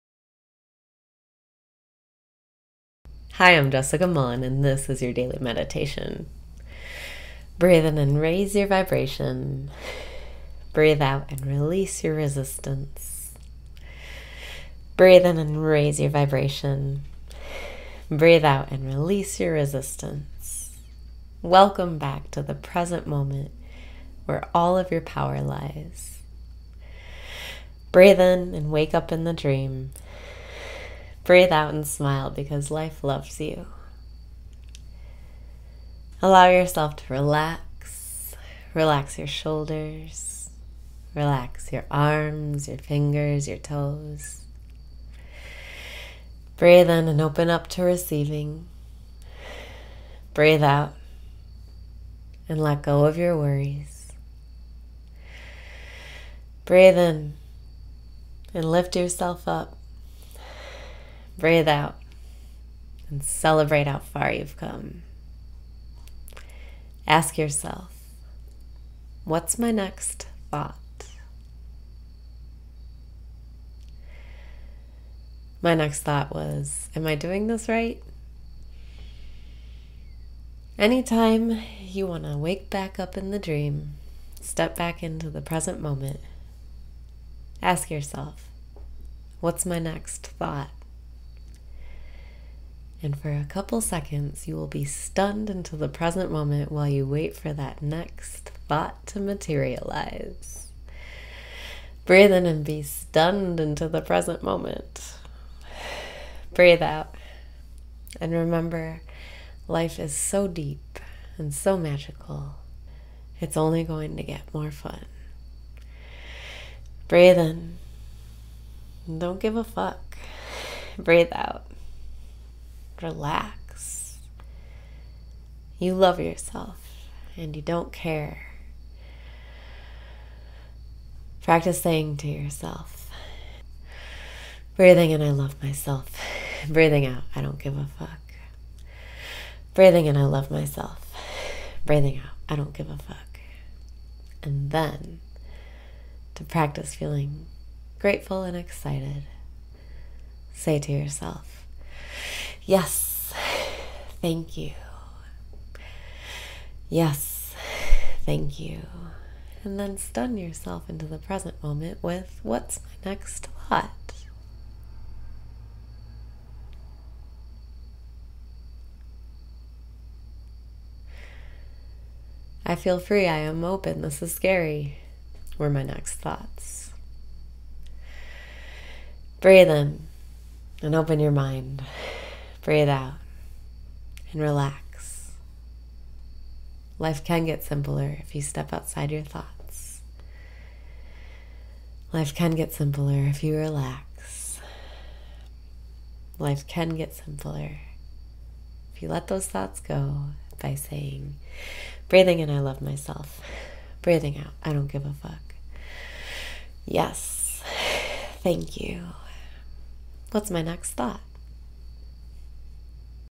Daily Meditation - Stun Yourself Into the Present Moment
Breathe out and smile because life loves you!

Allow yourself to relax. Relax your shoulders. Relax your arms, your fingers, your toes.

Ask yourself, “What’s my next thought?” My next thought was, “Am I doing this right?” Any time you want to wake back up in the dream, step back into the present moment, ask yourself, “What’s my next thought?” For a couple seconds, you will be stunned into the present moment while you wait for that next thought to materialize.

Breathe in and be stunned into the present moment
Breathe out and remember life is so deep and so magickal. It’s only going to get more fun.

Breathe in and don’t give a fuck!

You love yourself, you don’t care. Practice saying to yourself,

Then, to practice feeling grateful and excited, say to yourself,

YES!

YES!

Then stun yourself into the present moment with “What’s my next thought?” “I feel free I am open this is scary!” were my next thoughts.

Breathe in and open your mind

Life can get simpler if you step outside your thoughts. Life can get simpler if you relax. Life can get simpler if you let those thoughts go by saying

YES!

What’s my next thought?